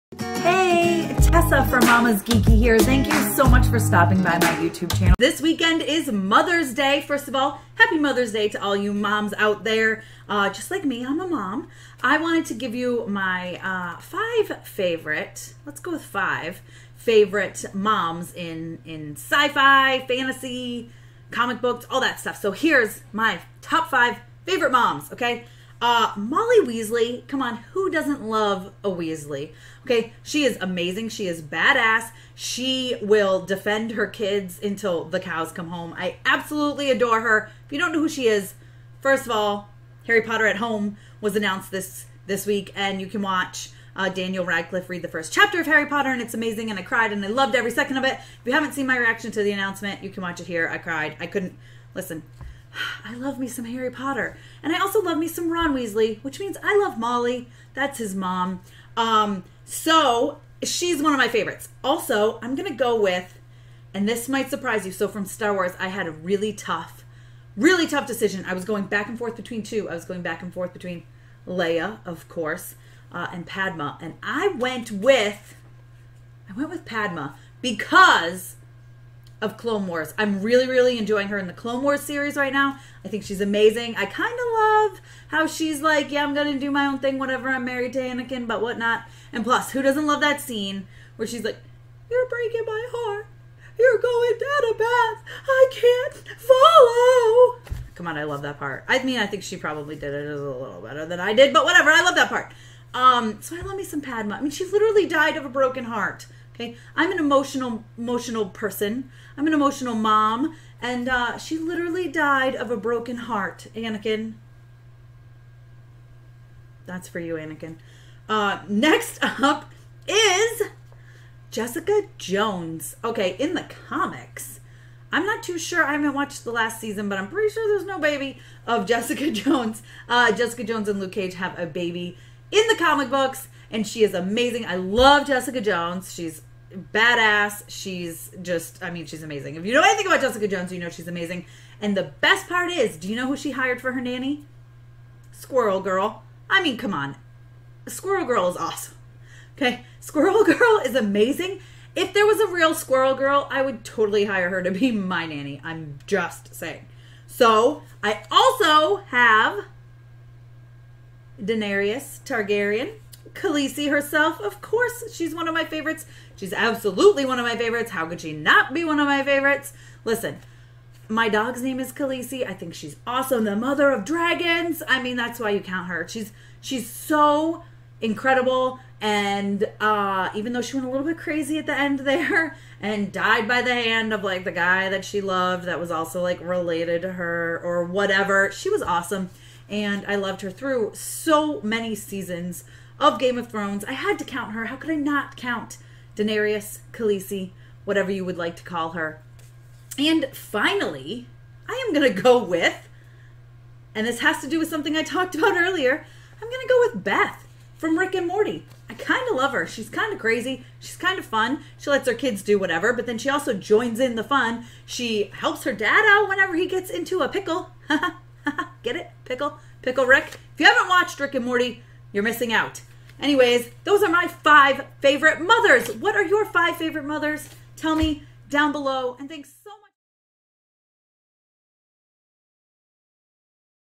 My 5 Favorite Moms In All Of Geekdom: Happy Mother's Day!
In this clip of The Mama's Geeky Show, here are my 5 favorite moms in the sci-fi, fantasy, geeky genre! What are yours?

Tessa Smith
Mama's Geeky